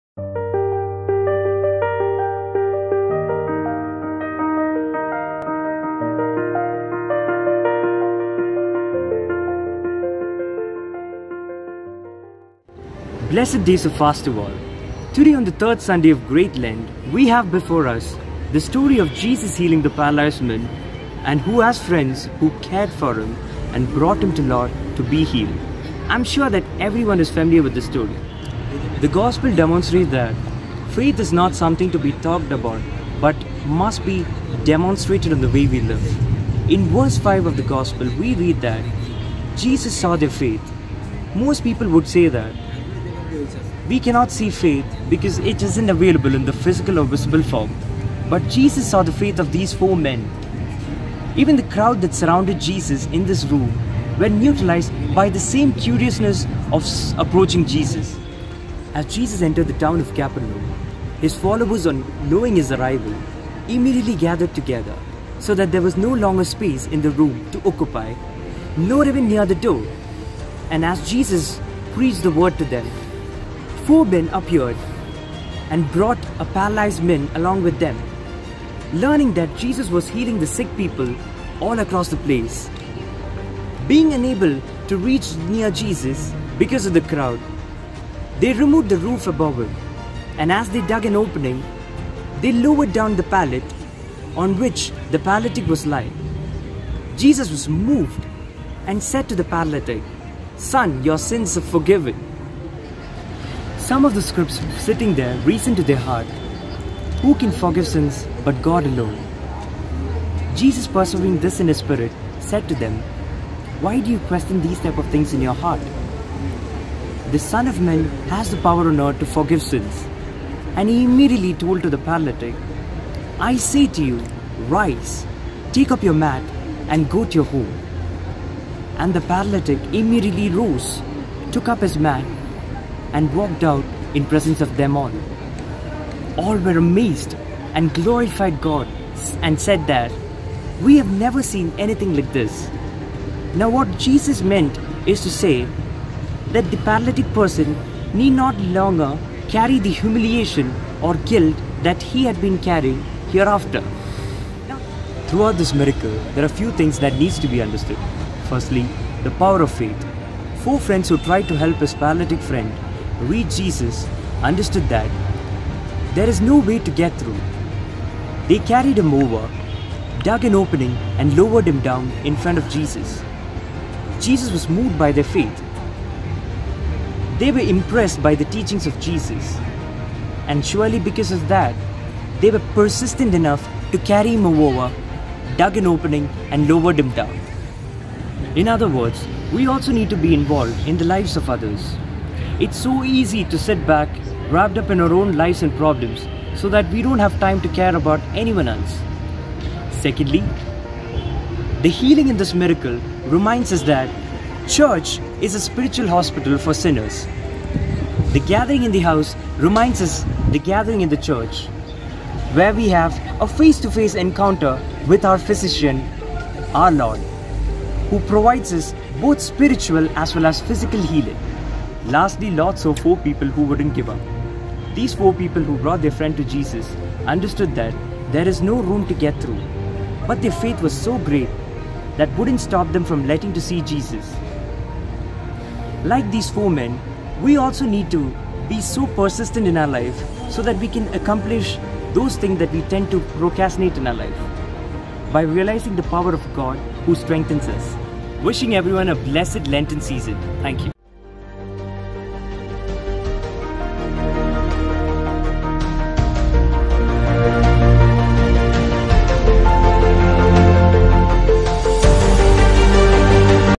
M'SHARIYO SUNDAY | SUNDAY SERMON(ENGLISH) | MAHIANA MGOCSM
Sunday Sermon
Third Sunday of Fifty days Lent (M'Shariyo Sunday)
Speaker: Milen Zachariah John
IV year BBA LLB

Let us know your feedback and suggestions in the comments below.

God bless